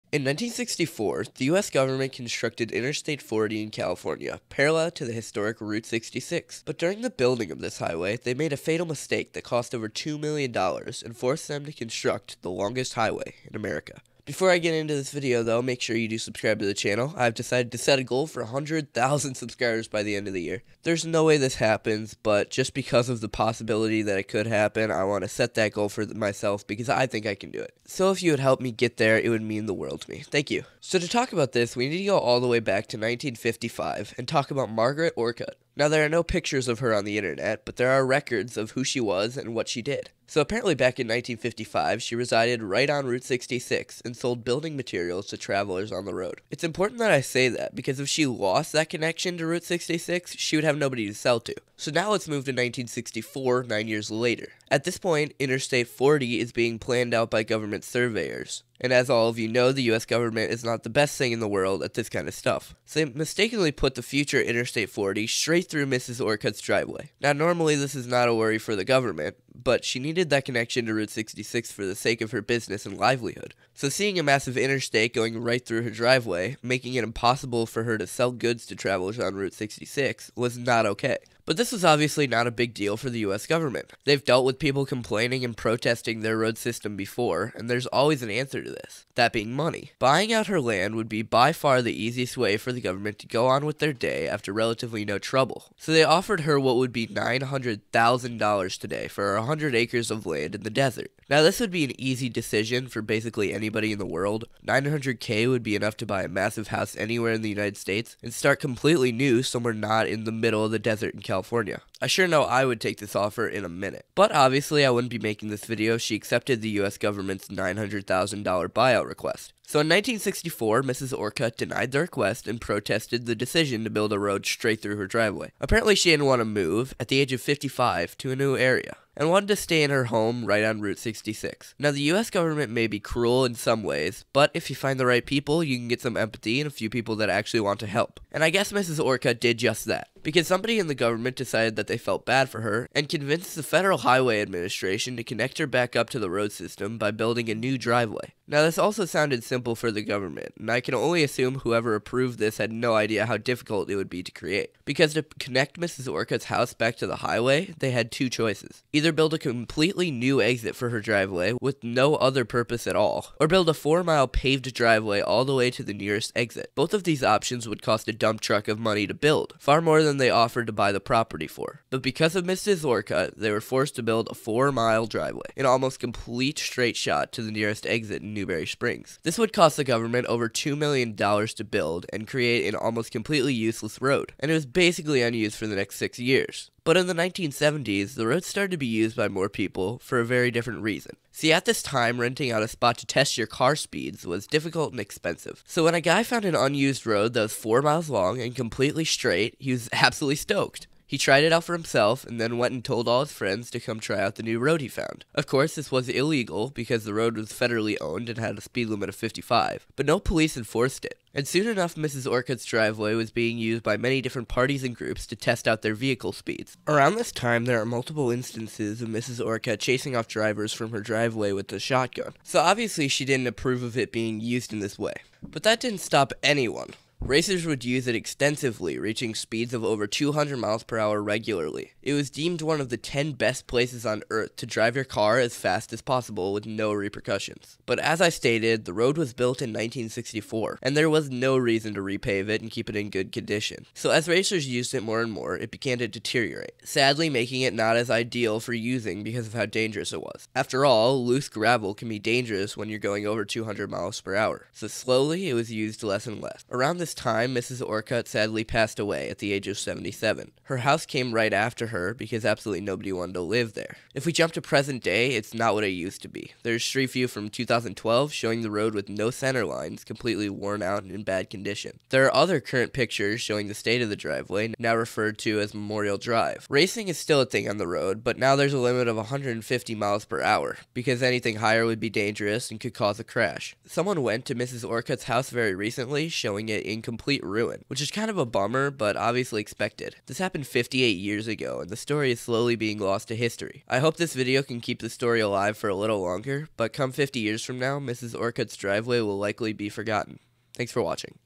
The Longest Driveway In America - Mrs. Orcutt’s Driveway
✵ In 1964, The US Government constructed Interstate 40 in California, parallel to the historic route 66. But during the building of the highway, they made a fatal mistake that cost them over 2 Million dollars and forced them to construct the longest driveway in America.

✵ Apply To Join The Video Team And Help With Future Videos!